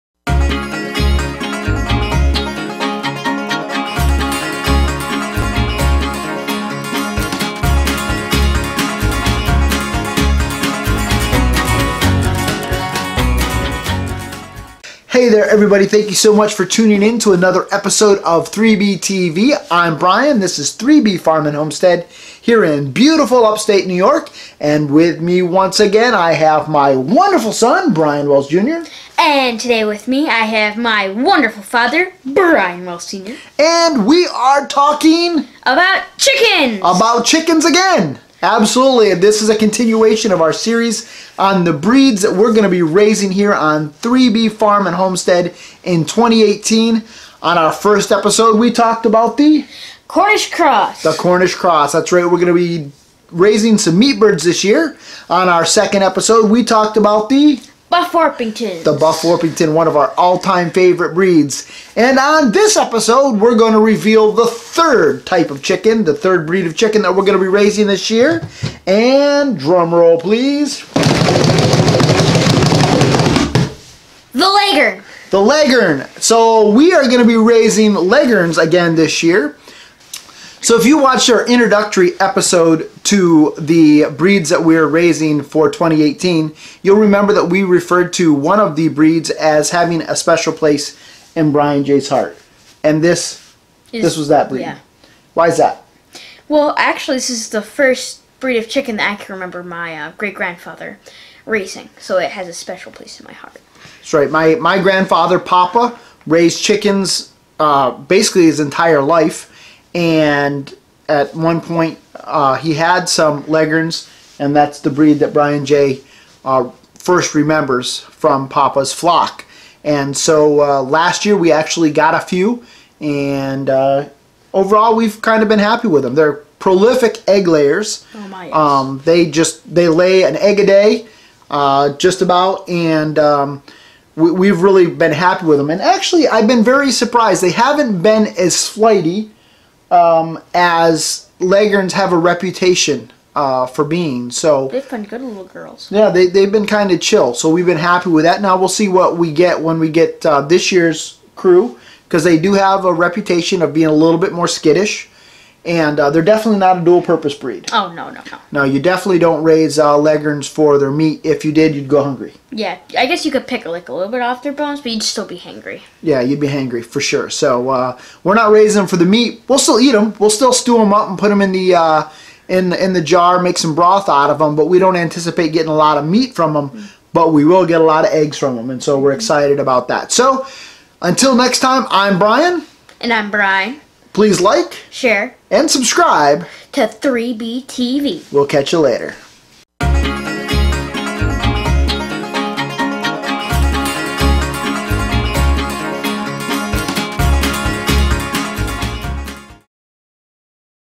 2018 Chicken Breeds: Breed 3
In this episode of 3B TV, we reveal the third of the six breeds we will be raising this year on 3B Farm and Homestead.